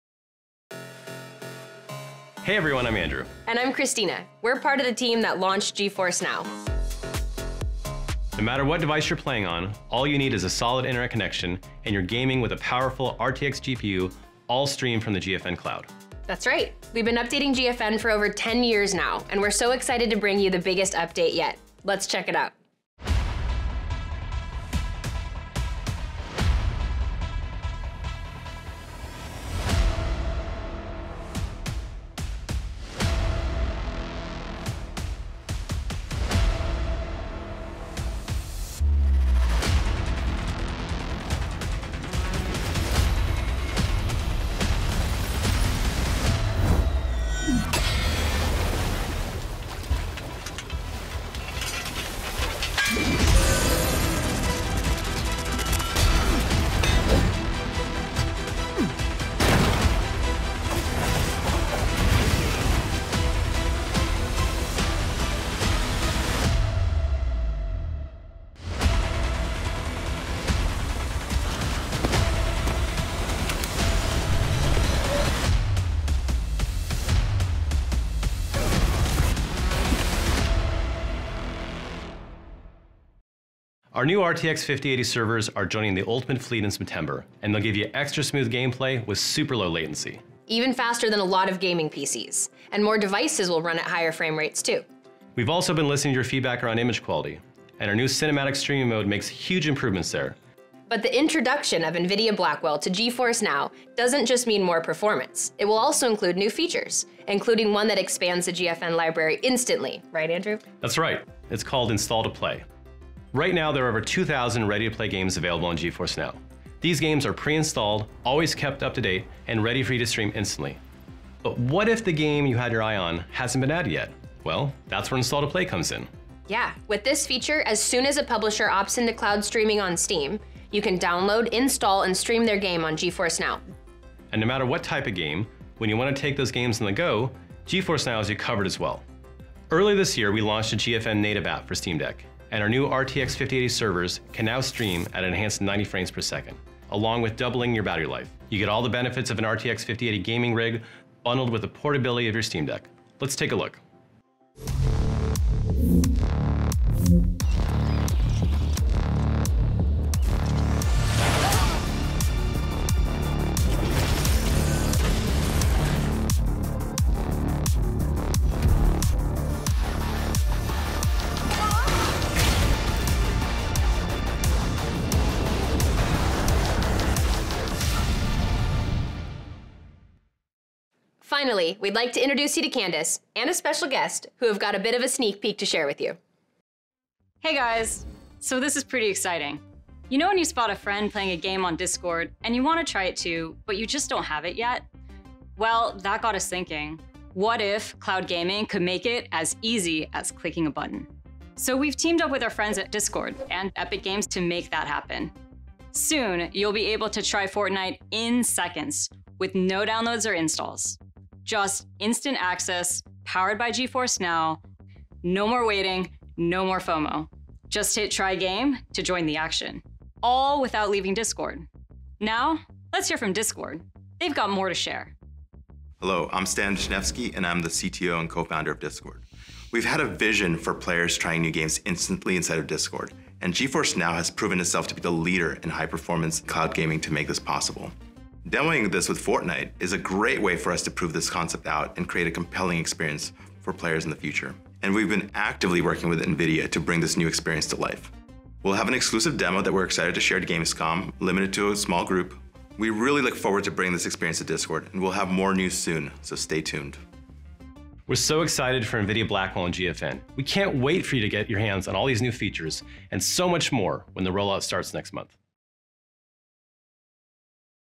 GeForce NOW | Announcements From GeForce On
Catch all the big news from GeForce On—new games, new features, and NVIDIA Blackwell RTX in the cloud.

Announced today, GeForce RTX 5080-class GPUs will be joining the Ultimate fleet, delivering a major performance jump that feels just like gaming on a high-end PC. Ultimate members will experience performance enhancements like a 2.8x increase in frame rates, new Cinematic Quality Streaming mode, DLSS 4 and up to 5K 120 FPS streaming, just to name a few. Plus, all Premium members will soon get access to Install-to-Play and cloud storage, a new way to multiply your library with over 2200 more Steam games. Check it out. Server upgrades start rolling out September 2025.